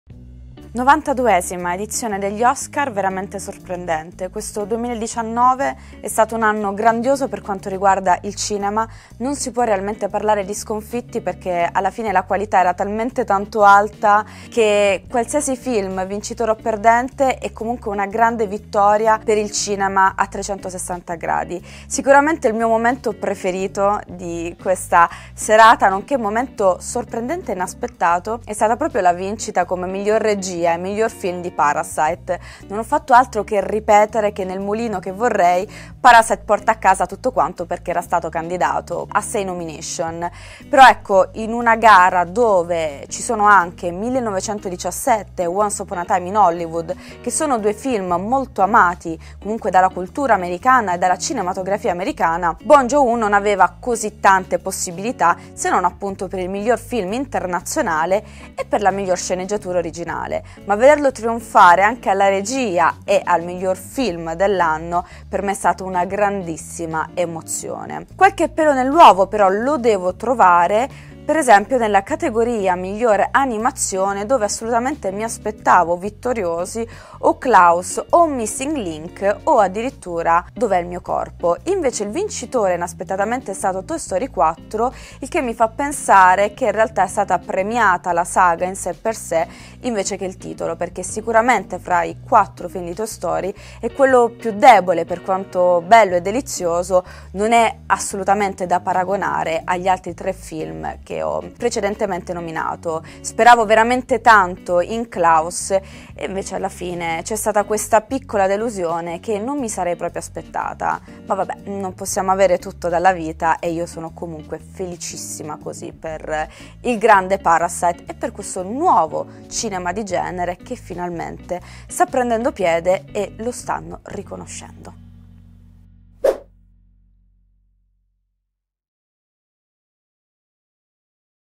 Oscar 2020 - Top & Flop di Gabriella Giliberti
Abbiamo chiesto ai presentatori della nostra maratona Oscar, andata in onda sul neonato canale Twitch di Movieplayer di raccontarci il meglio e il peggio della premiazione di quest'anno.

Ecco il parere di Gabriella Giliberti.